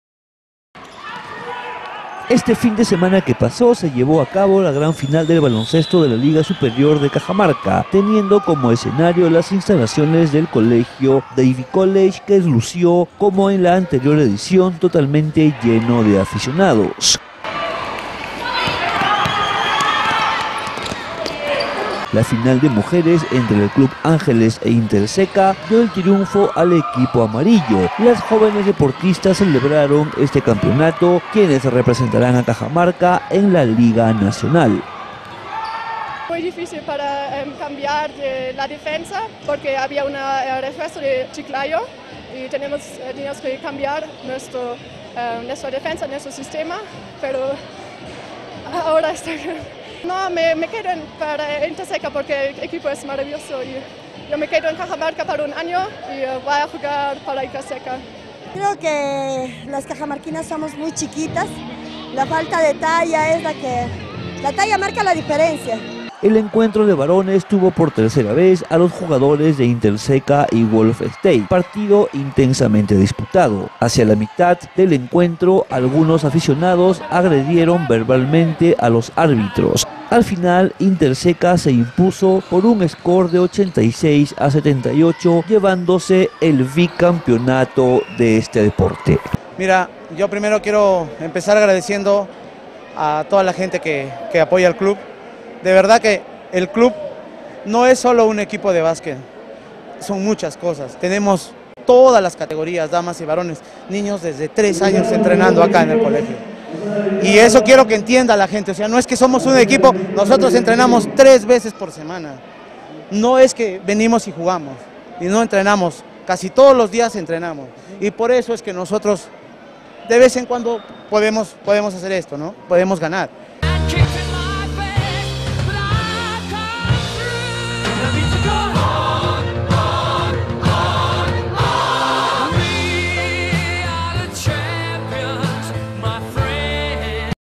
INTERCECA doblemente campeón en basquetbol
Este último fin de semana se llevó a cabo la gran final del baloncesto de la liga superior de Cajamarca, teniendo como escenario las instalaciones del colegio Davy College que lució como en la anterior edición totalmente lleno de aficionados.
La final de mujeres entre el club ÁNGELES e INTERCECA, dio el triunfo al equipo amarillo las jóvenes deportistas celebraron este campeonato, quienes representarán a Cajamarca en la liga nacional.
El encuentro de varones tuvo por tercera vez a los jugadores de INTERCECA y Wolf State, partido intensamente disputado, hacia la mitad del partido algunos aficionados agredieron verbalmente a los árbitros, al final INTERCECA se impuso por un score de 86 a 78, llevándose el bicampeonato.